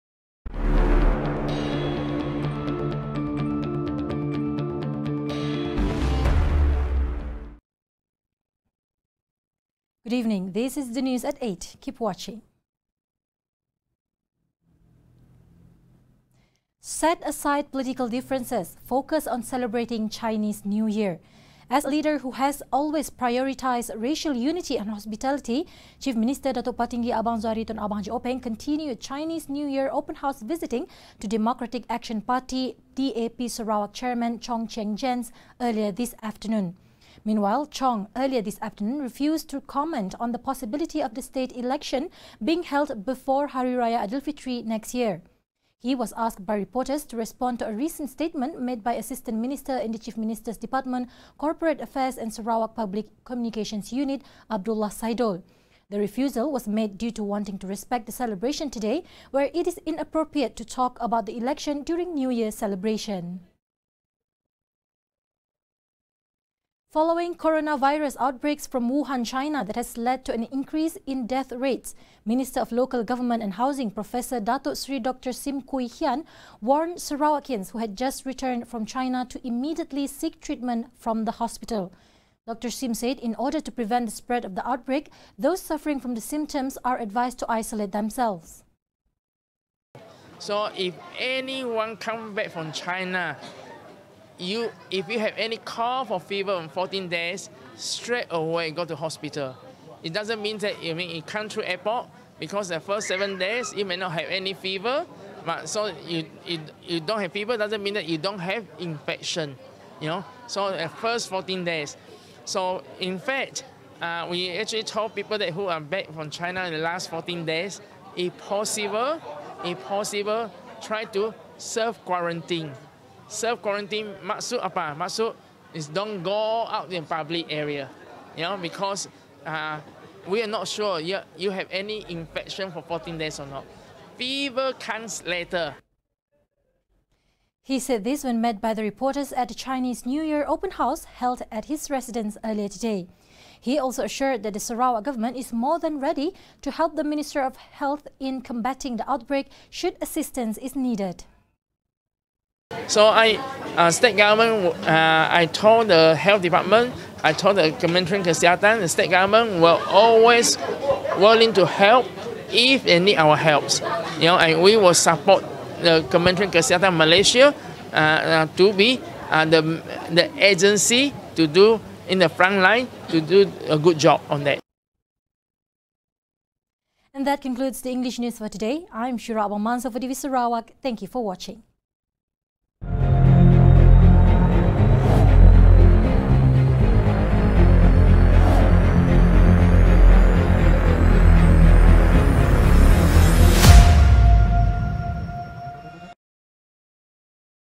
Set Aside Political Differences, Focus On CNY Celebration
1. Set Aside Political Differences, Focus On CNY Celebration

2. Sarawakians Returning From China Reminded To Seek Treatment At Hospital